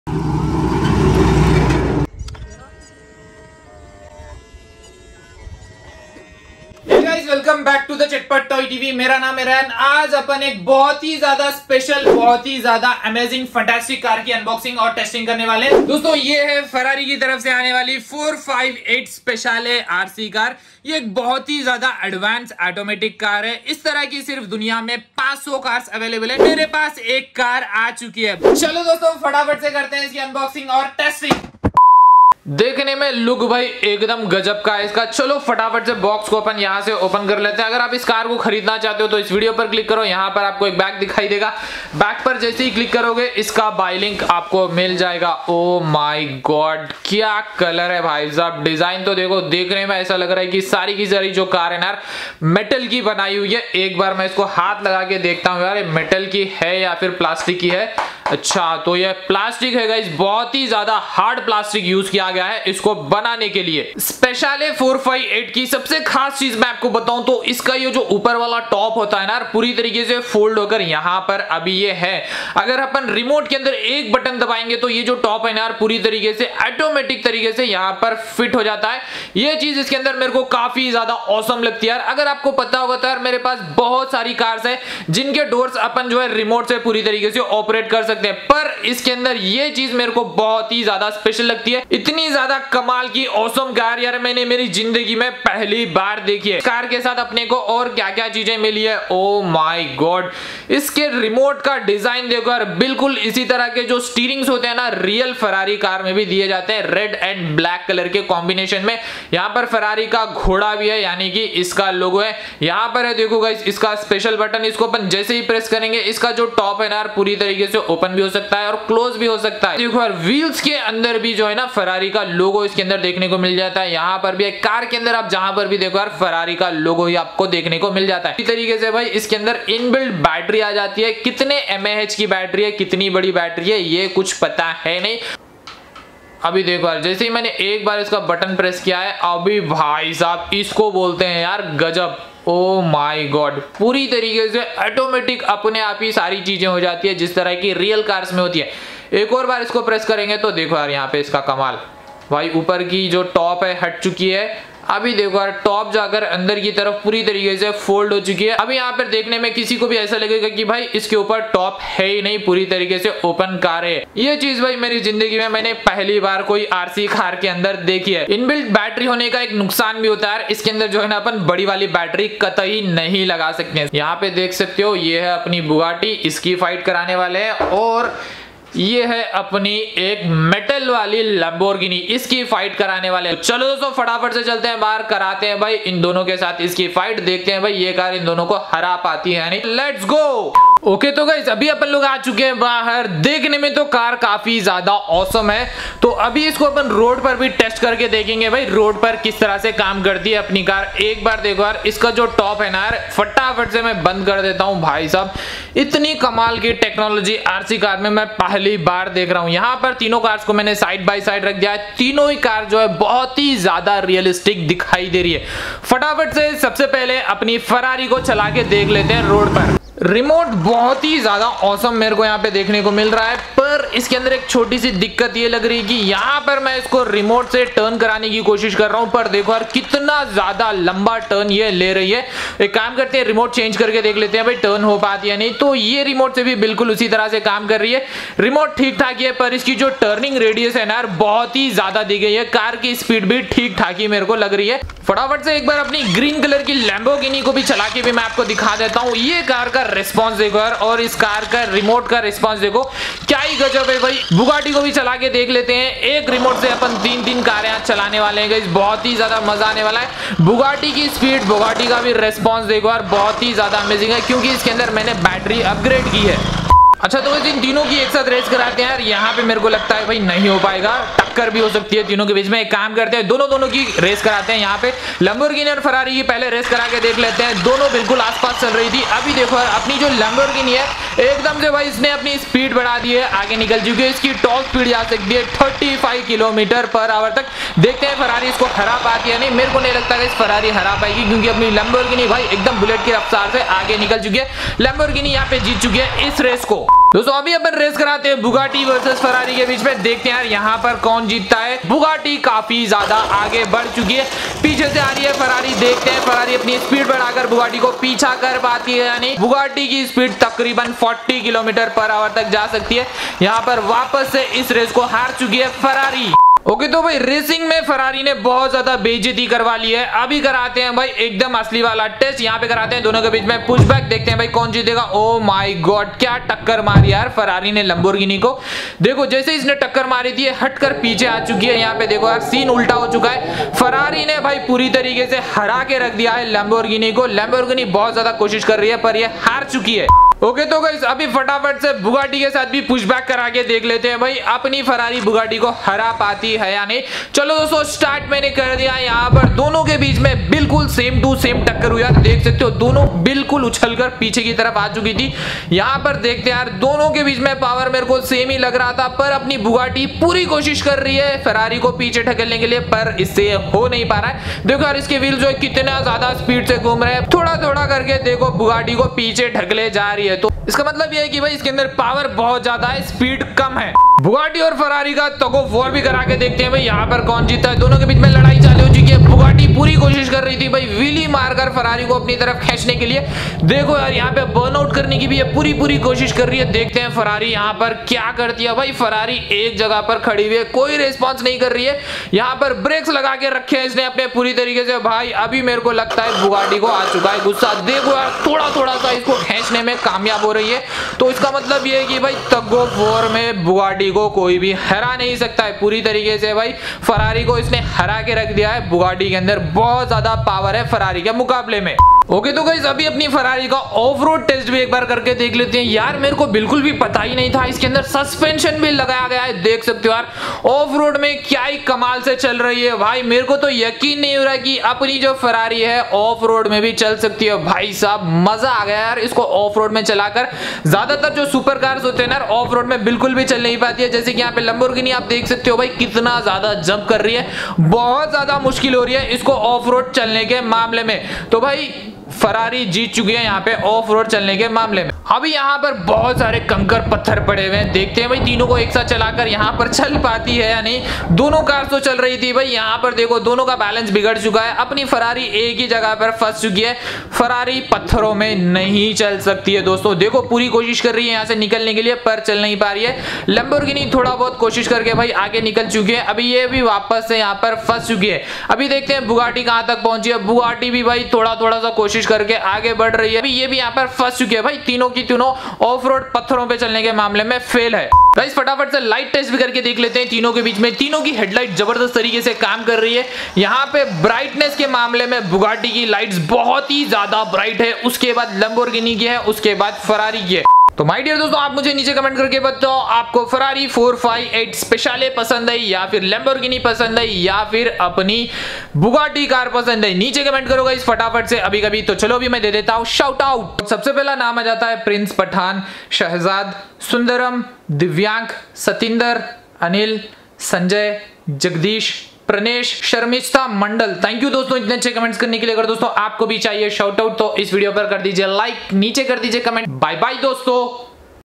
High Speed RC Ferrari 458 Speciale Car Unboxing & Testing- Chatpat toy tv
Dosto aaj ki video mai maine unbox aur test kiya hai Rc Ferrari 458 speciale car Yeh rastar special limited edition real scale model remote control car hai iska top fully automatic remote cse control ho sakta hai